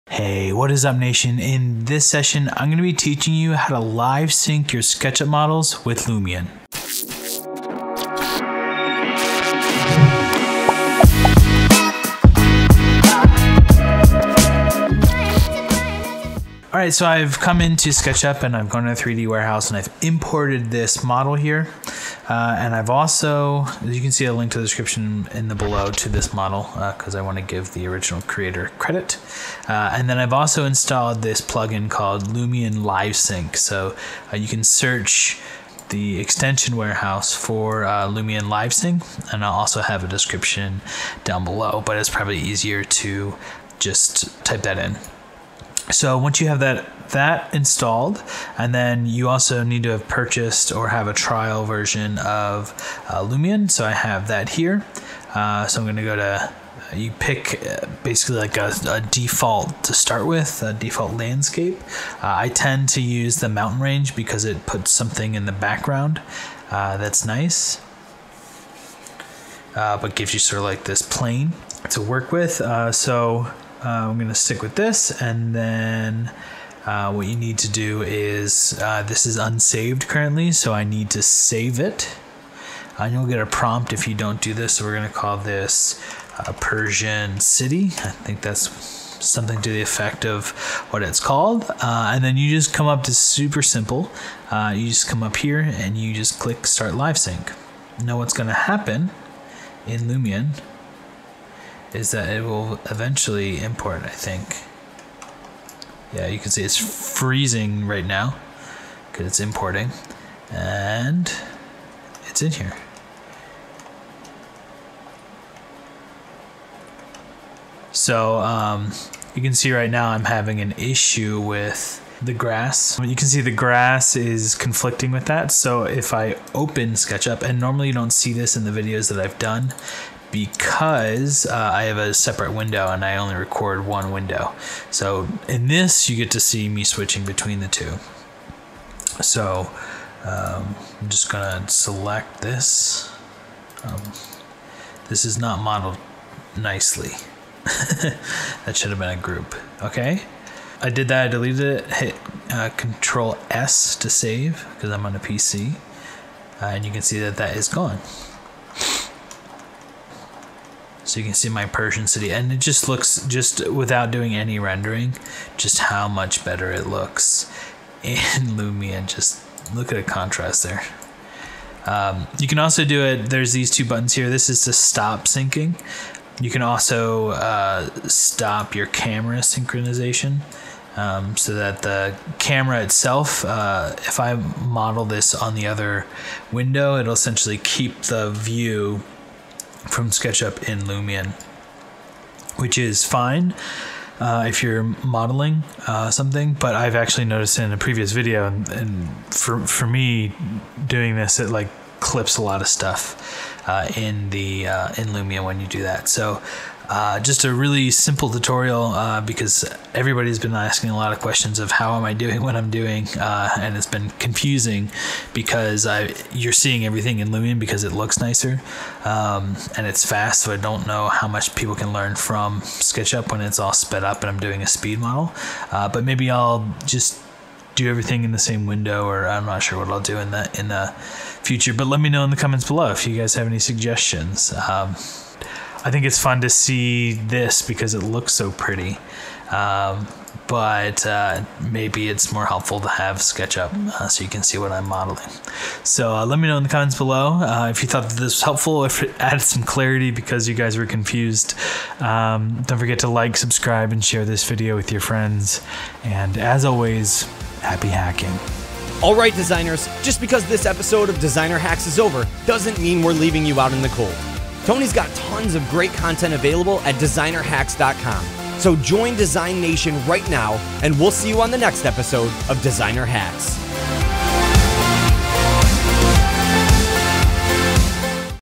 How to Sync Sketchup with Lumion
In this sketchup tutorial we cover how to sync sketchup with Lumion.

Software & plugins we use in this video:
Sketchup 2018
Lumion LiveSync for Sketchup
Lumion 8

3d model used in this video is called "Persian City" created by Jonathan G.: